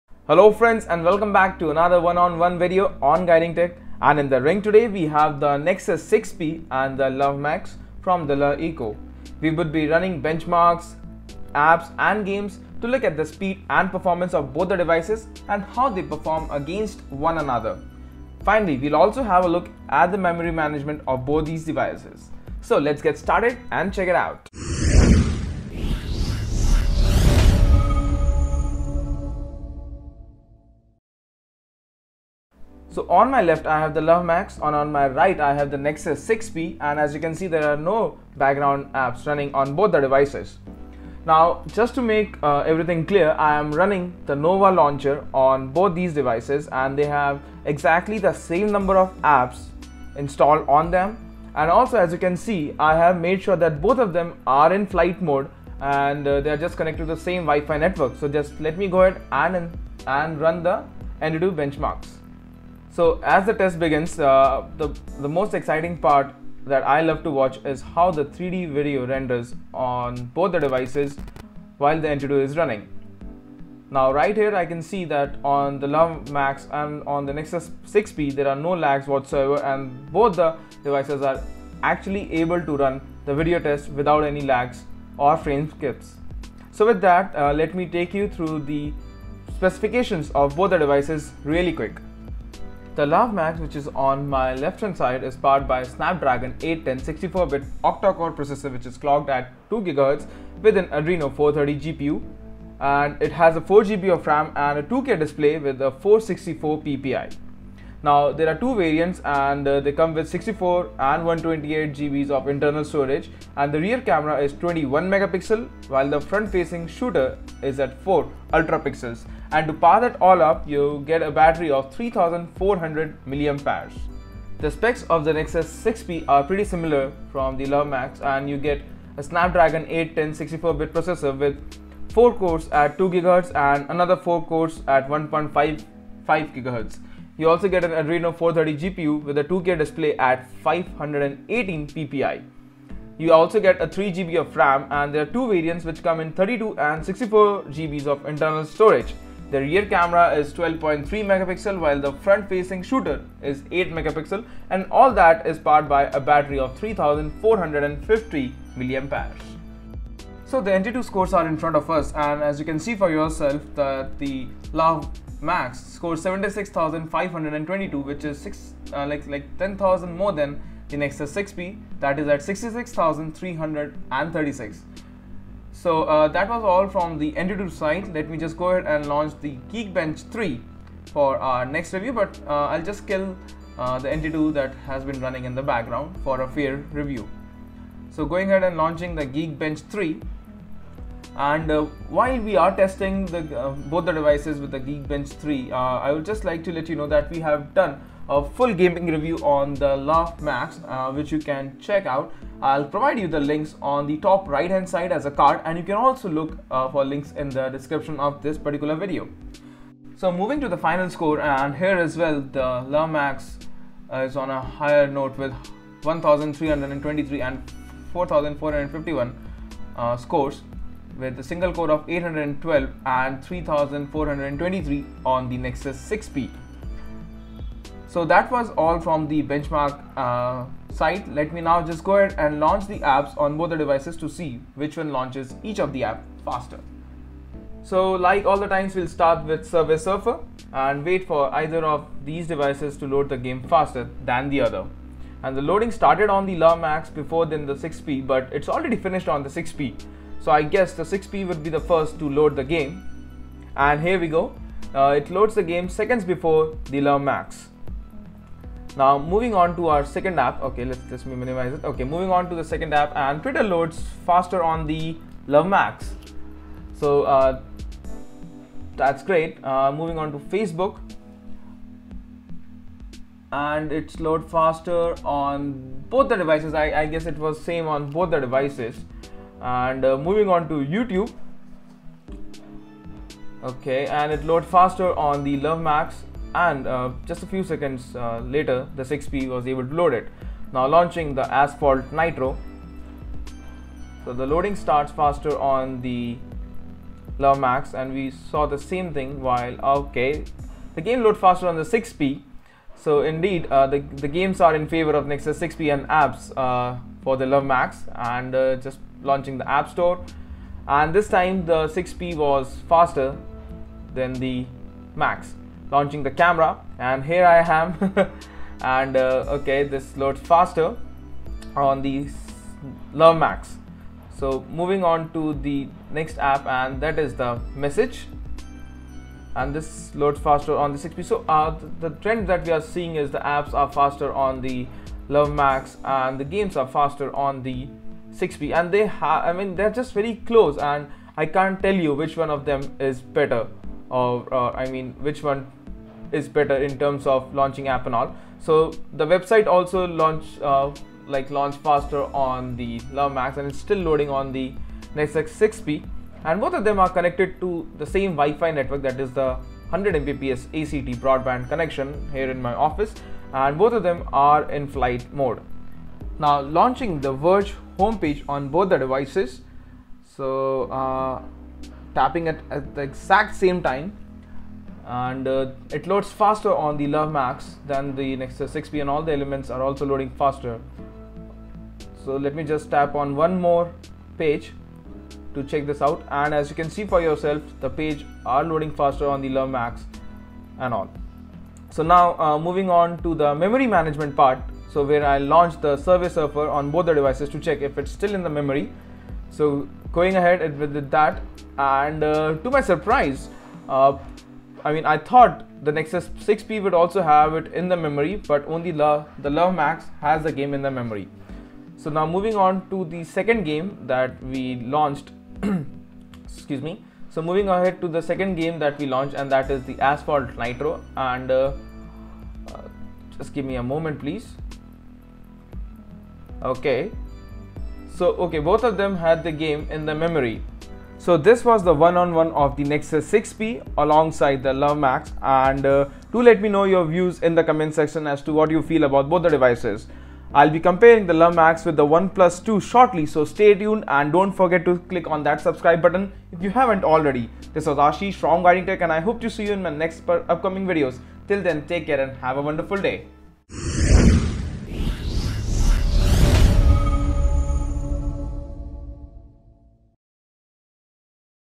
Le Max vs Nexus 6P - Speed, Benchmark and Performance test | Guiding Tech
A quick benchmark and speed test comparison between LeEco Le Max and Nexus 6P.

First we will start with benchmark scores on both the devices and later we would be launching some games and apps to have a one on one between the devices and then take a look at the memory management.